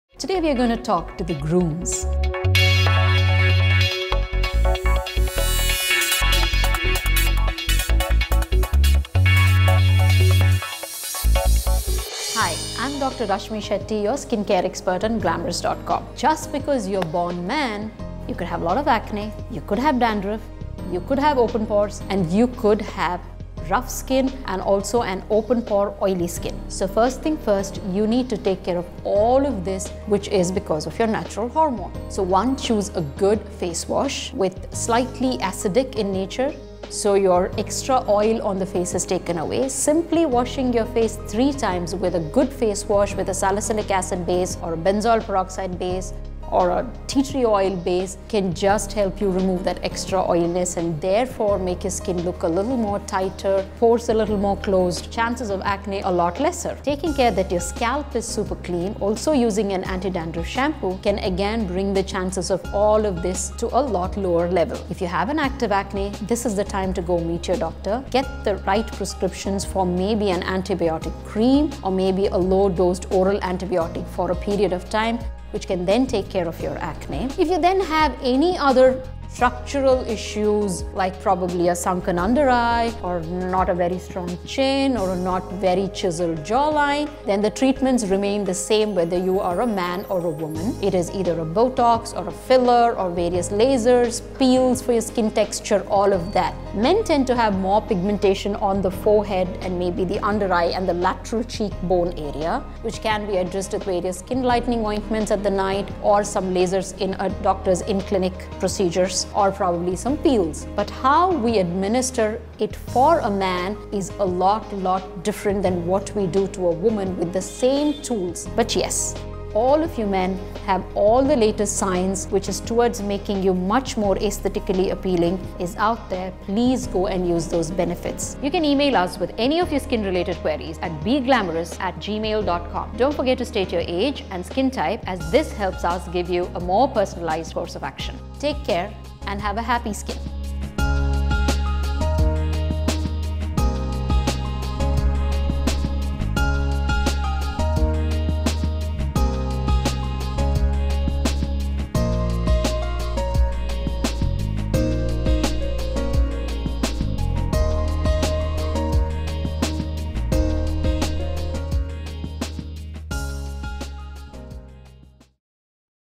4 ESSENTIAL SKIN CARE TIPS FOR GROOMS
It is as important for the groom to have great skin on his wedding day, as us brides-to-be.  So here's our Skincare Expert, Dr. Rashmi Shetty with 4 SKINCARE TIPS FOR THE GROOM - a must-see video with essential advice for your man, to keep him looking his best on your wedding day.